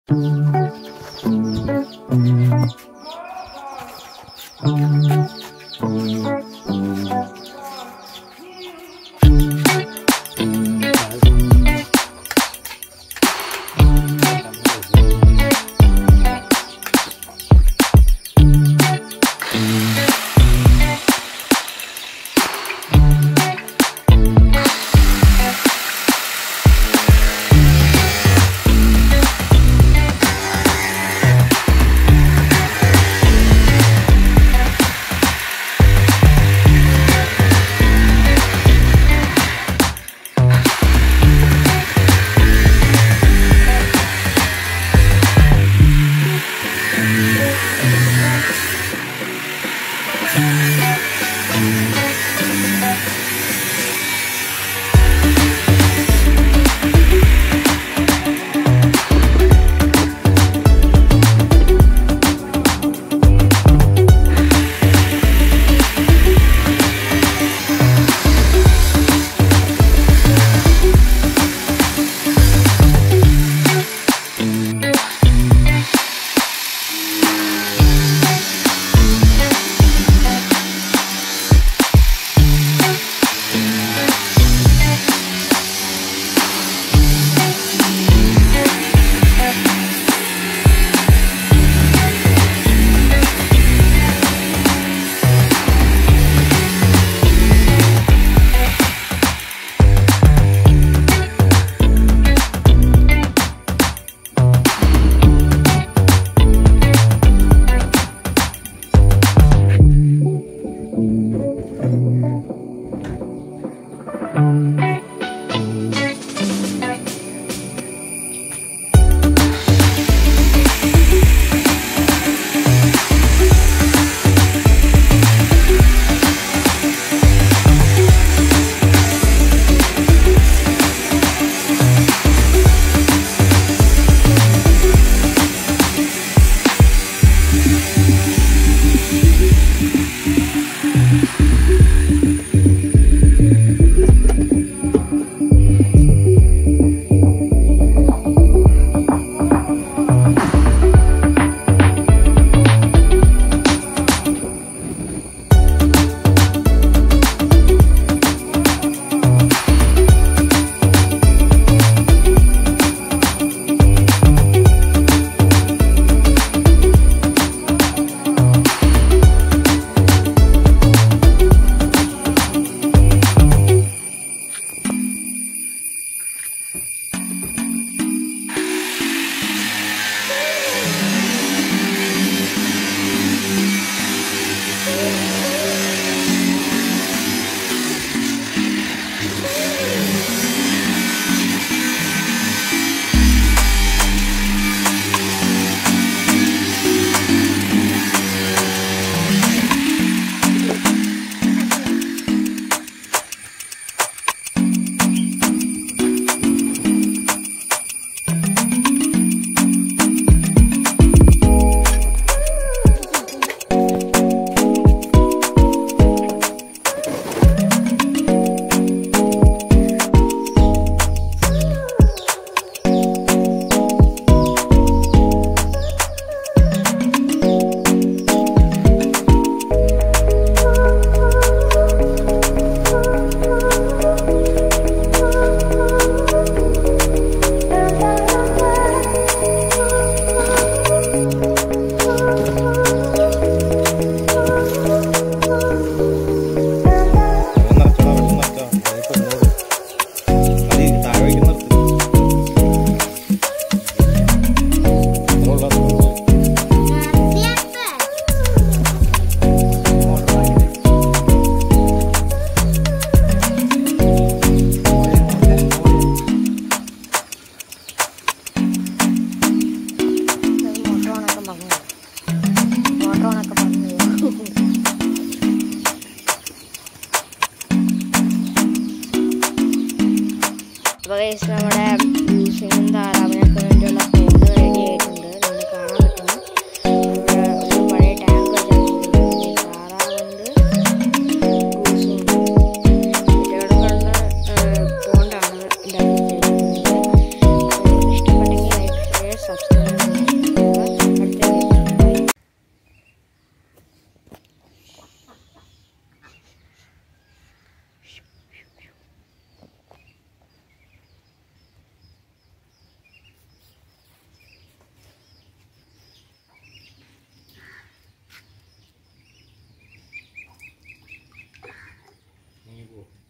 How to make a pond for duck , goose, etc / out of old water tank
old is gold. in this video I'm going to say how to make a pond  I hope u all like it